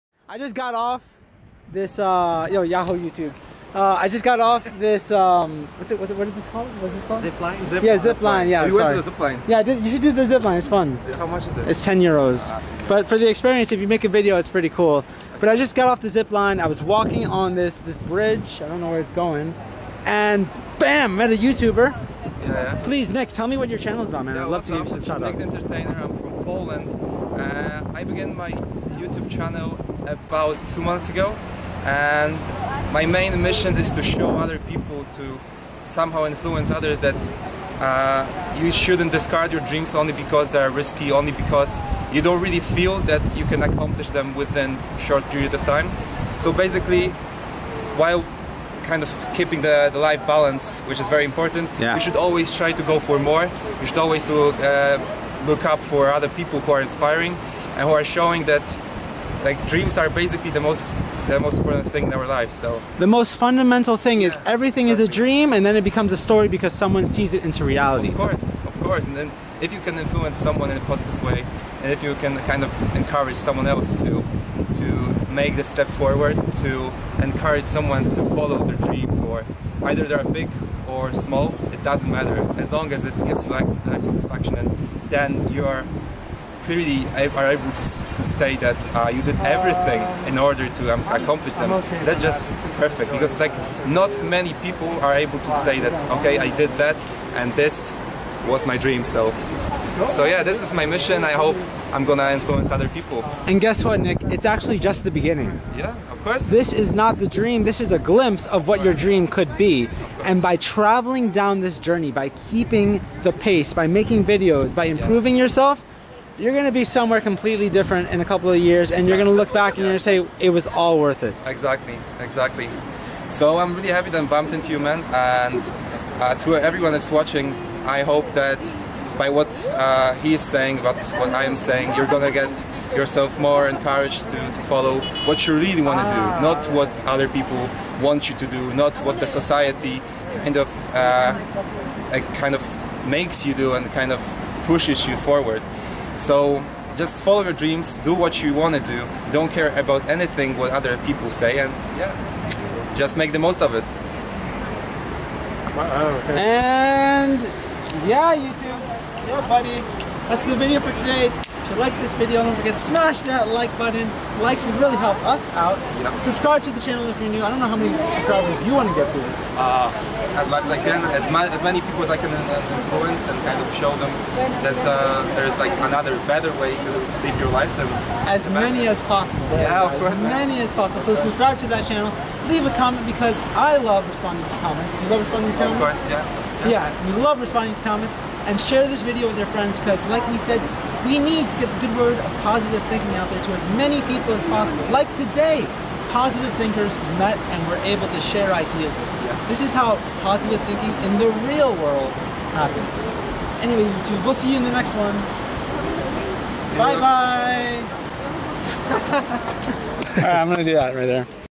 Meeting People in Toledo: Nick the Entertainer
Still hyped up from the zip lining, I was shouting " America F Yea" on this bridge and my friend said, "If that guys an American hes going to smile."

He smiled, which promoted me to ask him where he was from and little did I know I was getting to meet another youtuber!

This is how positive thinking works in the real world yeah?